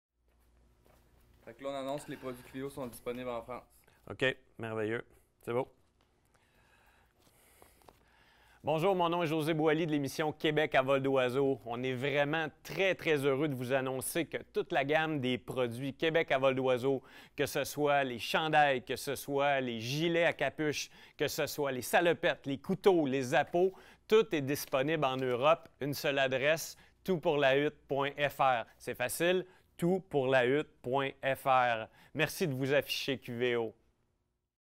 Québec à Vol d'Oiseaux et José Boily présente Toutpourlahutte
Toutpourlahutte.fr devient le distributeur officiel des produits dérivés de l'émission Québec à Vol d'Oiseaux diffusé en France sur la chaine Chasse&Pêche et présentée par le désormais célèbre José Boily. Retrouvez tous les articles QVO